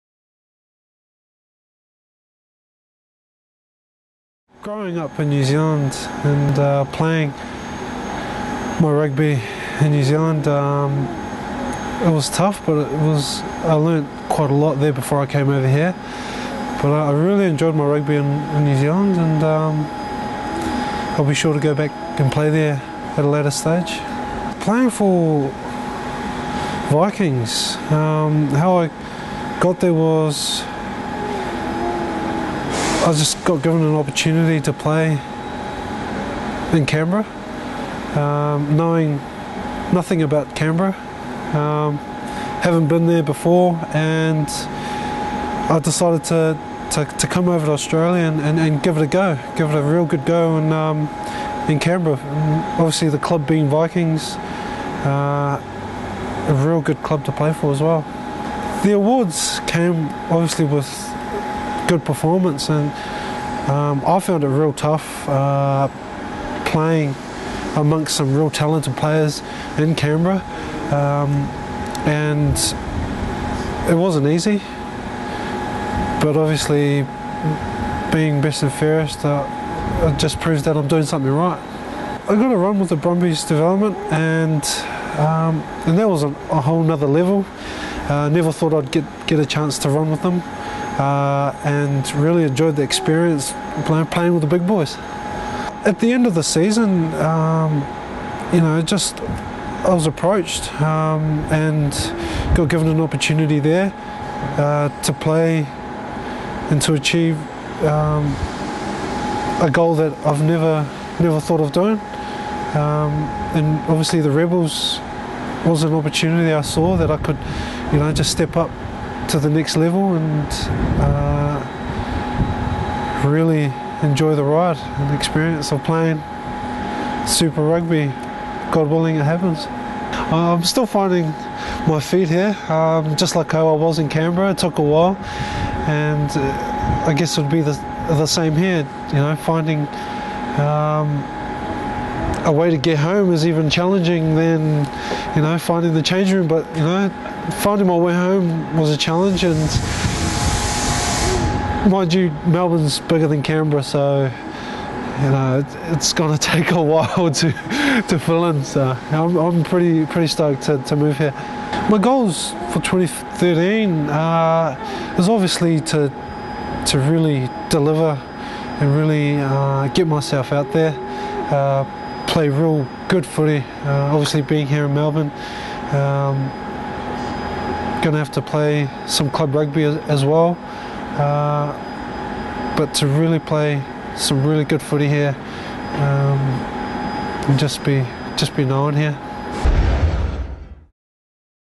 Introducing new RaboDirect Rebels recruit Pat Leafa
Pat Leafa joins the Rebels EPS squad following an outstanding season playing club rugby in Canberra. Leafa moved from New Zealand to Canberra in 2012, taking up a spot in the ACT Brumbies development side and playing for the Tuggeranong Vikings. The move was a great success for Leafa as he won the MacDougall Medal for the most outstanding player in the Canberra club competition.

Leafa is thrilled to move down to Melbourne and begin life with the Rebels, and is delighted to be able to work alongside somebody with a familiar sounding accent in Ged Robinson.